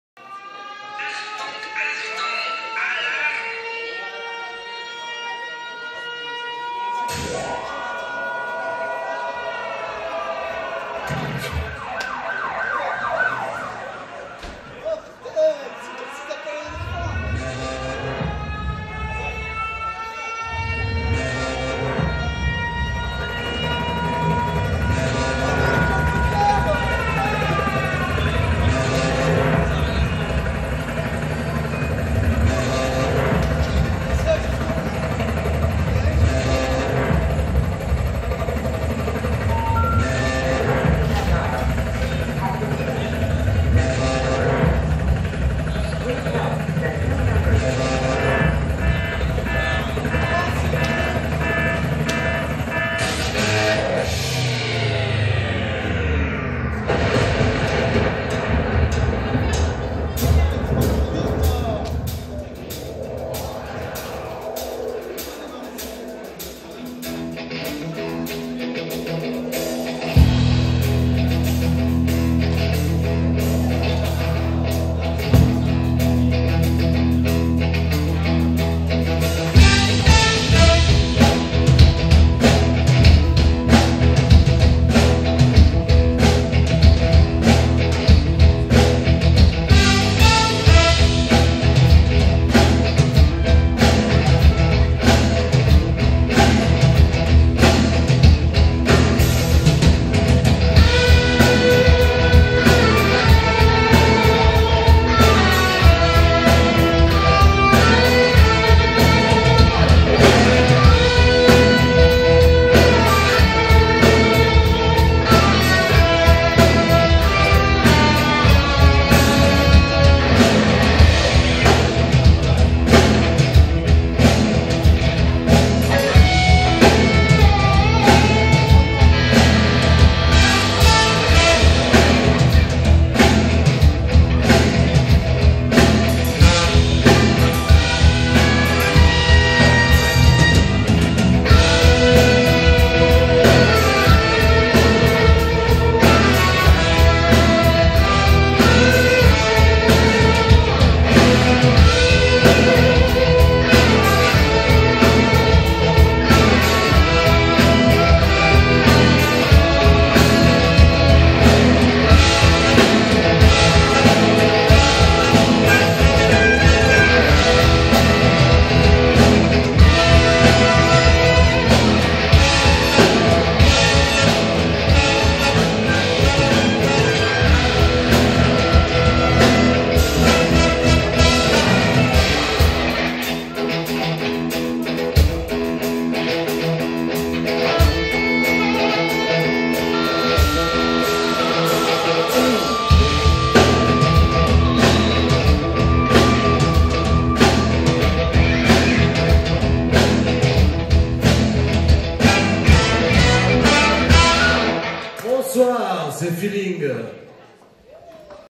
intro indicatif
Orchestre FEELING - indicatif début de soirée en salle des fêtes - 10/11/2013